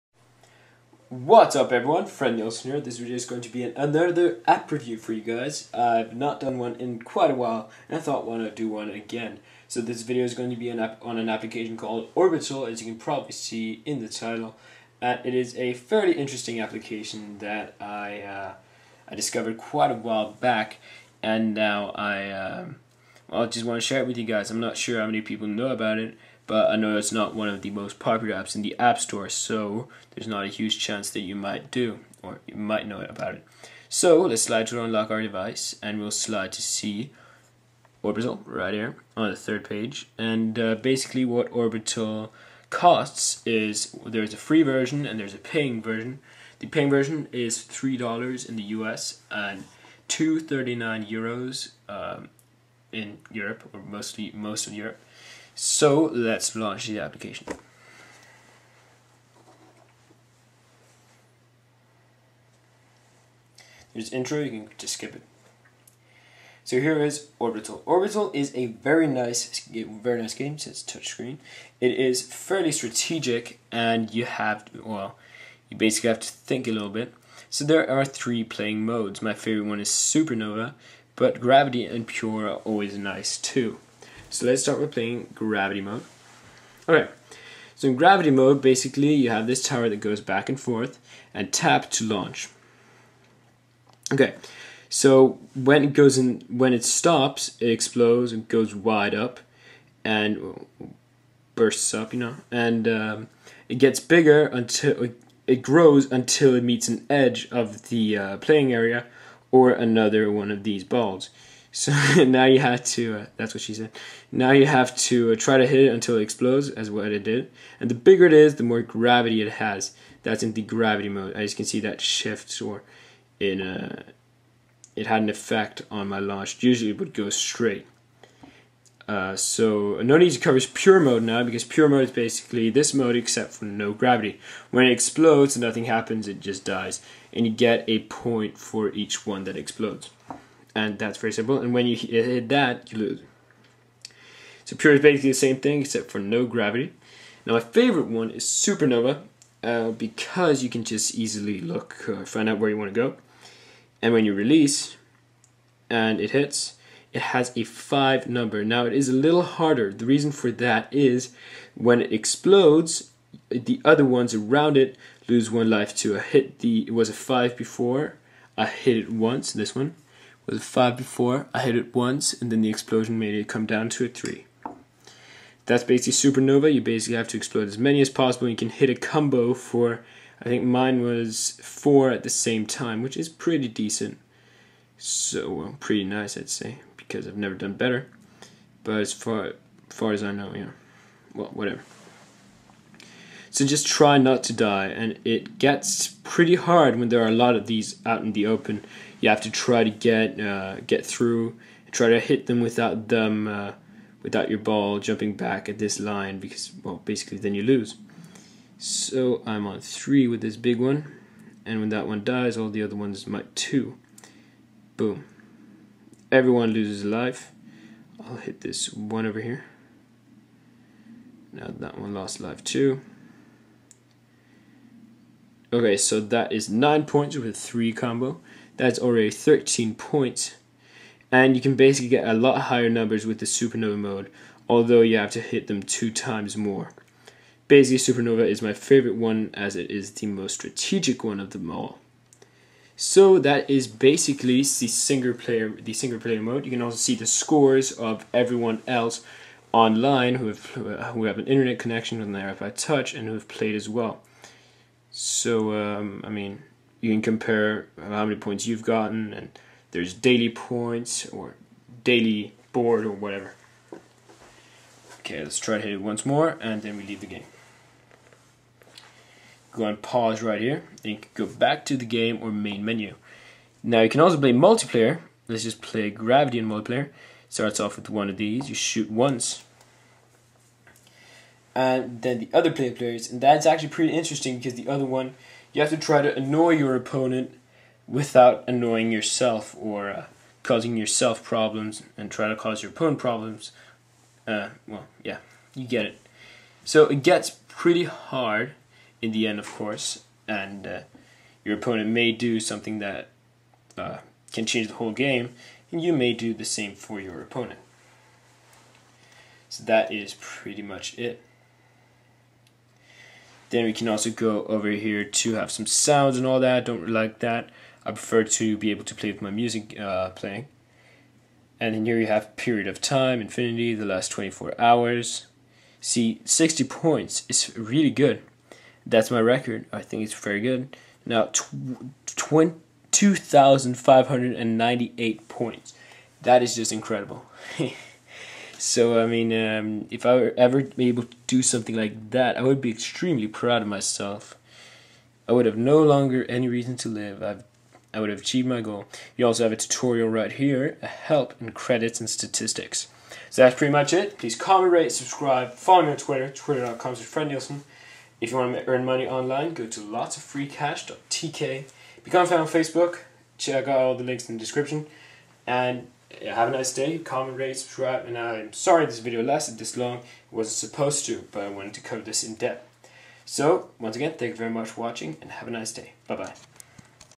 App Review: Orbital - Cool, Fun and Strategic
Orbital is an app I play when I have nothing better to do. It's quite fun actually. I would hands-down recommend this to anyone who enjoys a fun, simple and strategic app from time to time.

Make Lots of Money Online!